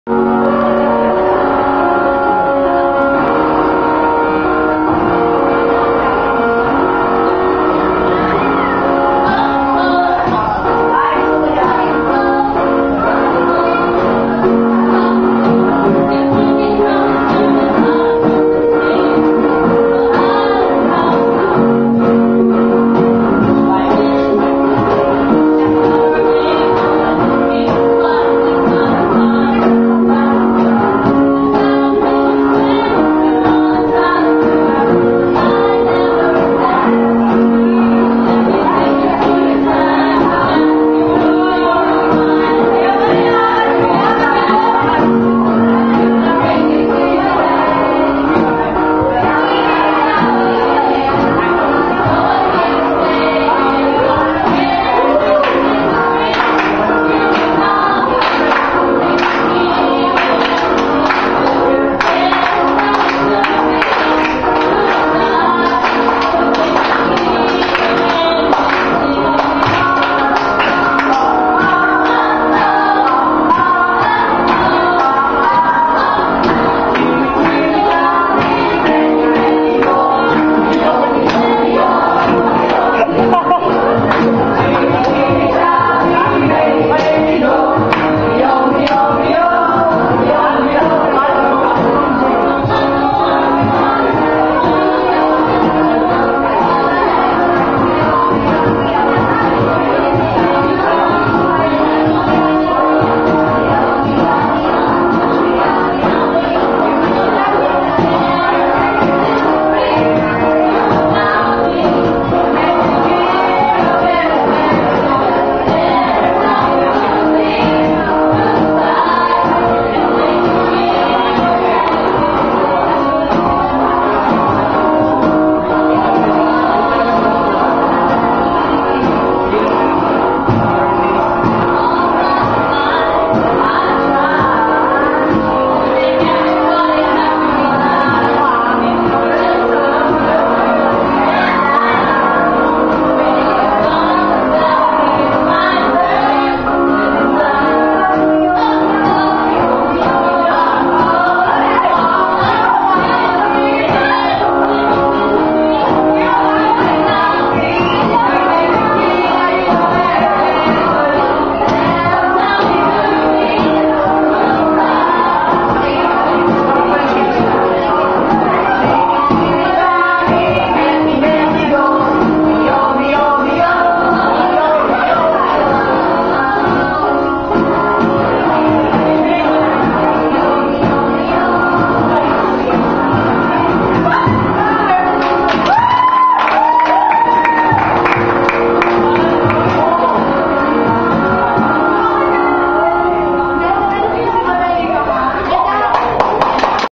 FridayNightTugtugan: cover performance 3
Last Friday Night Tugtugan of 2010, Kuya Cajo - mikestand, All Rights Reserved to the song that was covered in this video, I currently don't know the song please bear with me, Full Acknowledgement to the people playing in this video as I don't know your names po ^^ please bear with me also,